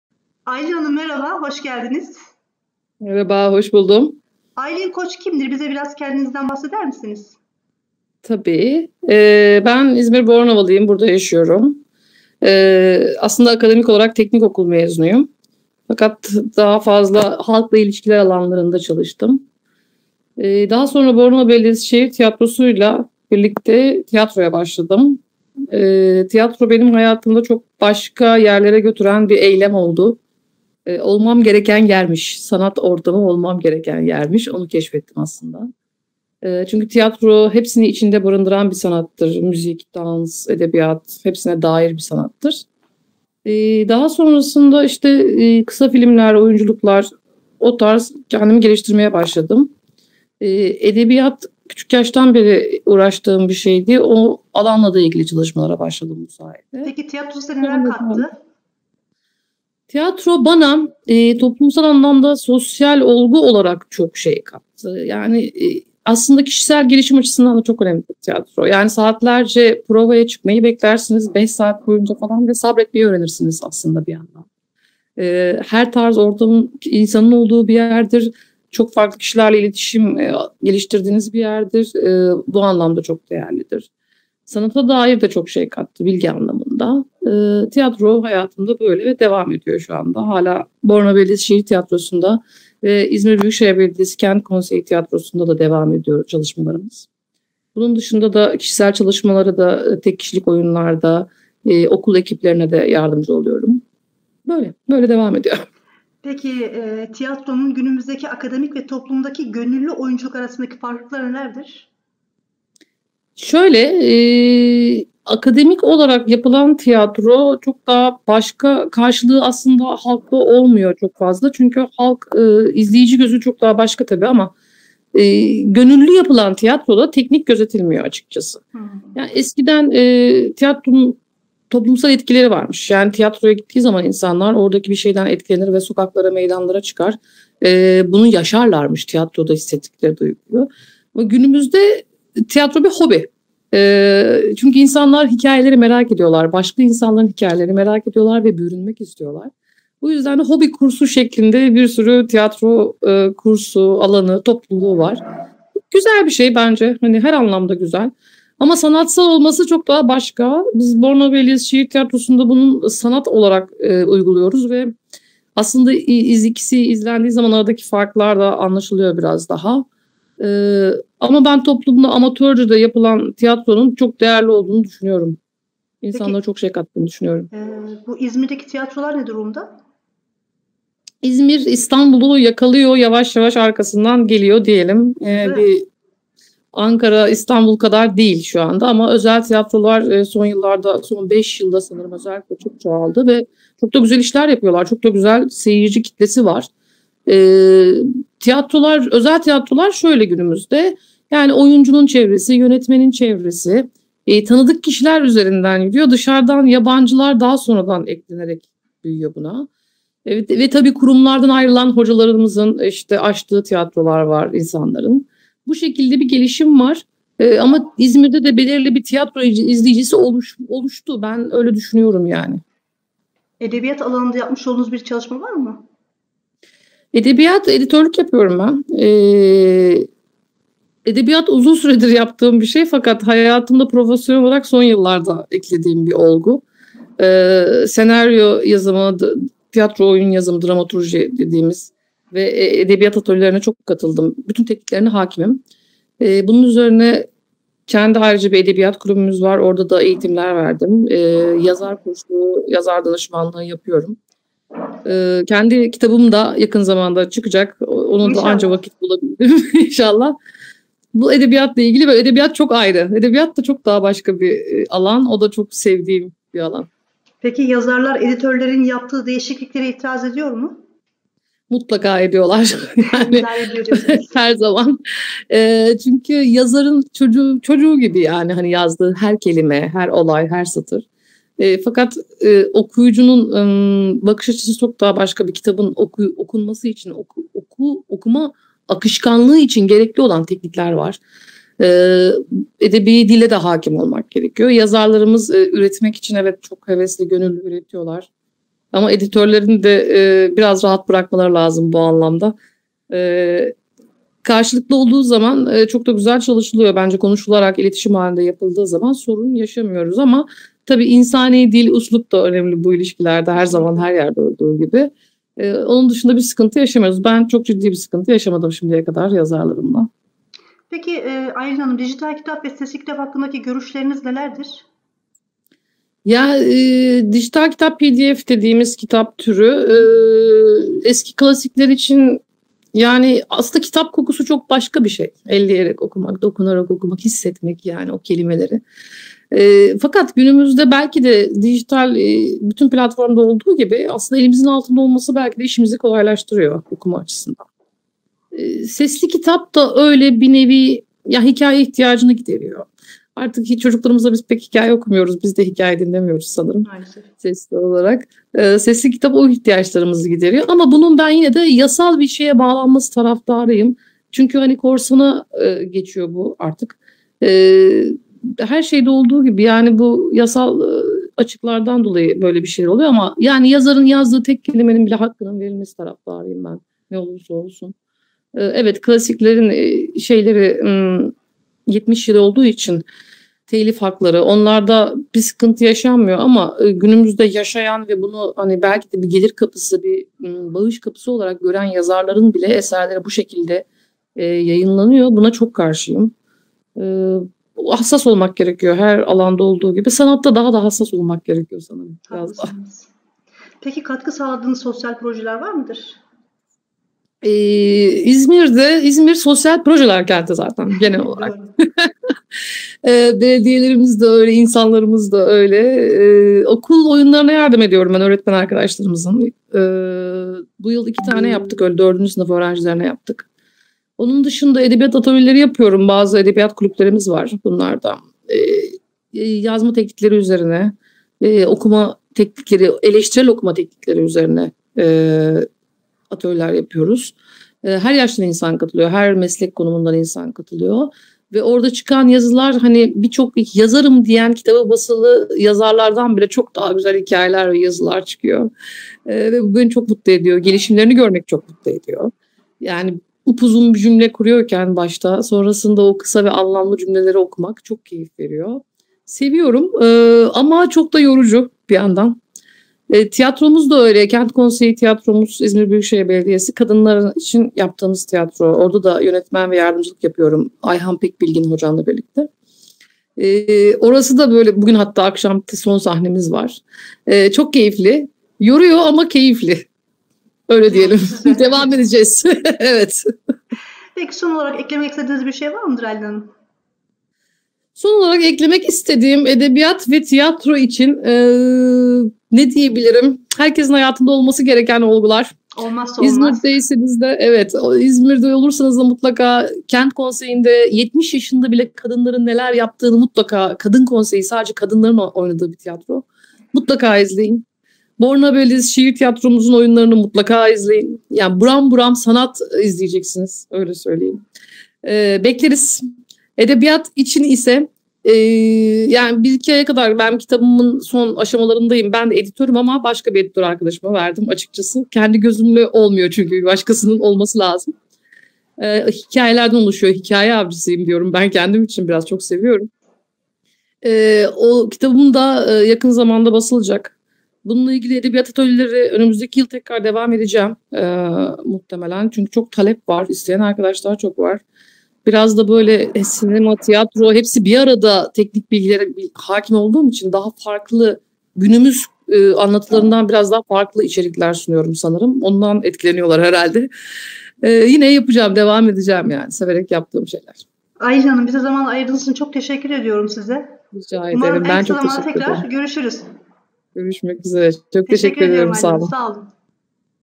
Seher Uyar Soruyor Yaşam Koçu Editör Aylin Koç Cevaplıyor
Aylin Koç Eğitimini burada tamamladıktan sonra sadece üniversite eğitimi için Afyonkarahisar’da kısa bir süre yaşamıştır. Akademik olarak teknik okul mezunudur fakat hayatı edebiyat müzik tiyatro ile dolu dolu geçmiştir.
Bornova Belediyesi Şehir tiyatrosunda uzun yıllardır devam eden birlikteliğiyle tiyatroya adım atmıştır. İzmir Kent Konseyi, Kadın meclisi bünyesinde de tiyatro yardımcı yönetmenlik ve oyunculuk yapmaya devam etmektedir.
Uzun yıllardır sürdürdüğü okuma ve yazma tutkusunu Tuhaf Kadınlar edebiyat kulübüyle birleştirip, araştırma inceleme ve yazıyoruz atölyeleriyle perçinlemiştir.
Senaryo yazma eğitimi, dramaturg oyun yazarlığı ve birçok edebiyat atölyelerini tamamlamış olup, yazar koçluğu ve geliştirici editörlük de yapmaktadır.